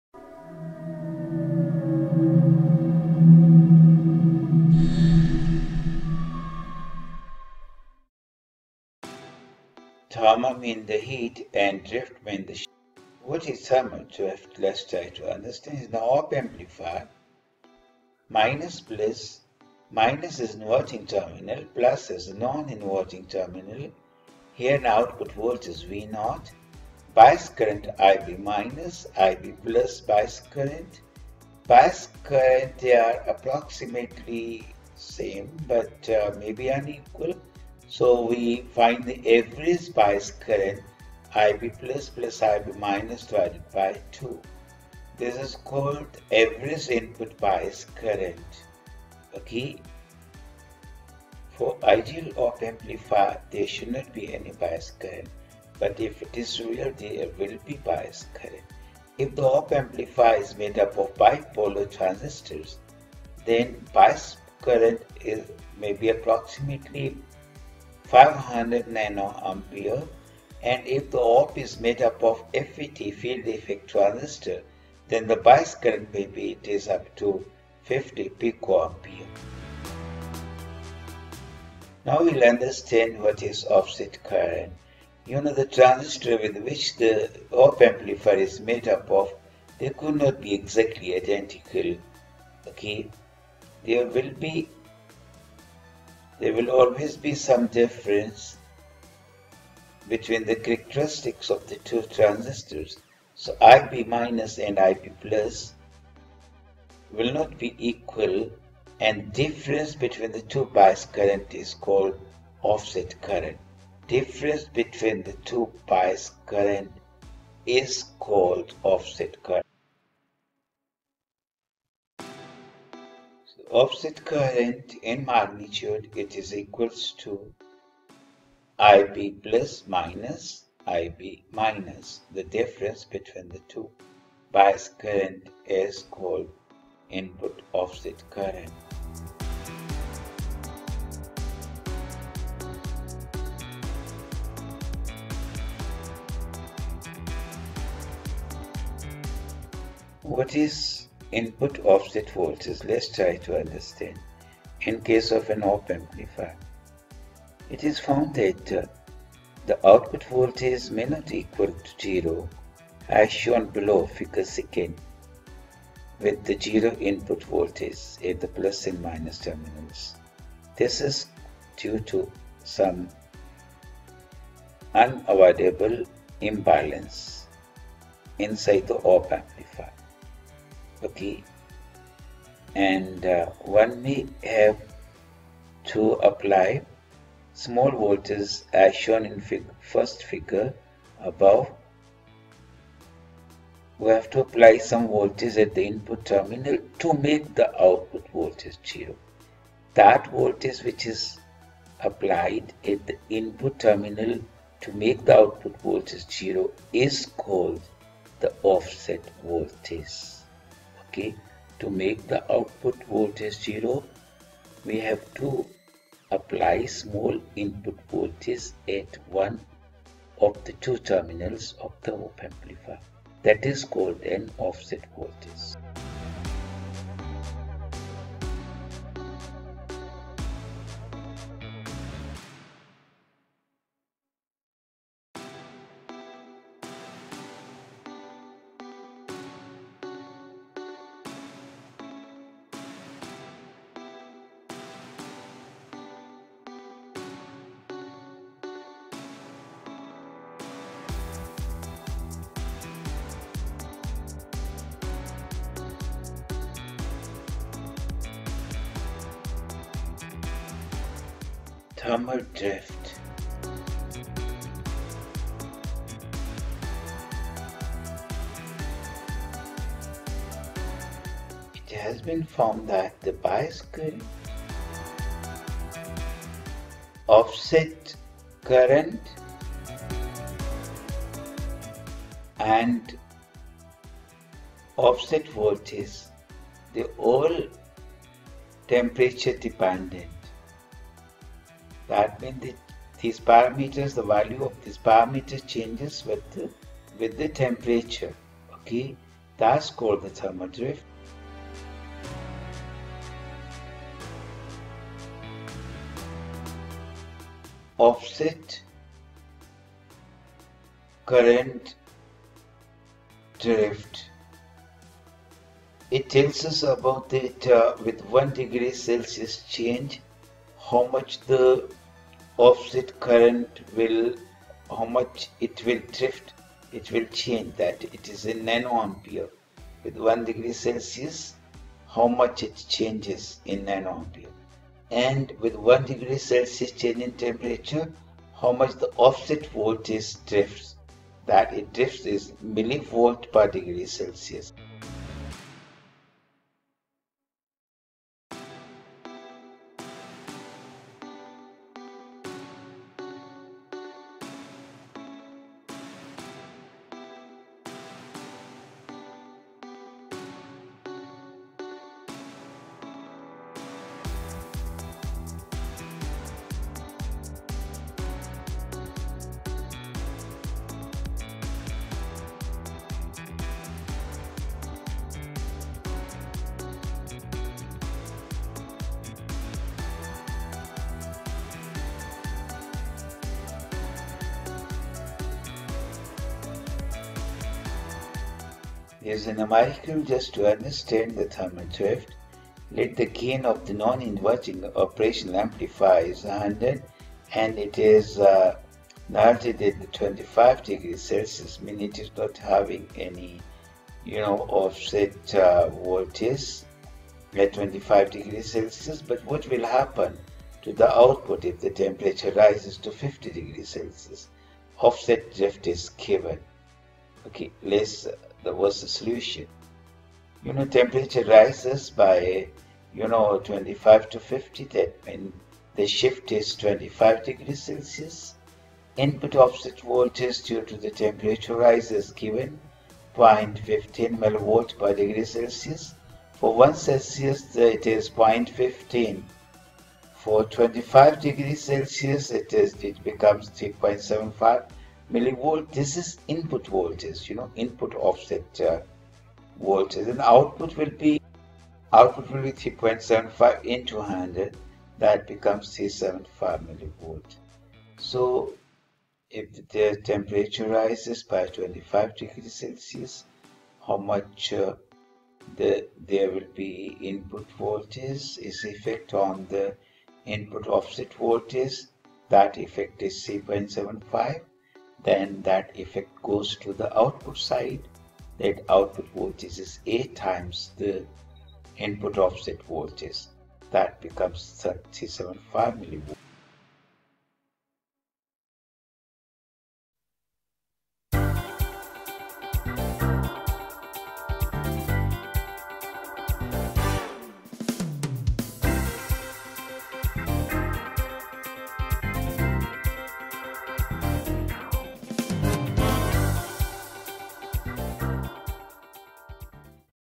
Difference Offset and Bias current, Thermal drift
The phenomenon of change in offset current and offset voltage , expressed in nA/degC and mV/degC is known as Thermal drift. You can find handwritten notes on my website in the form of assignments.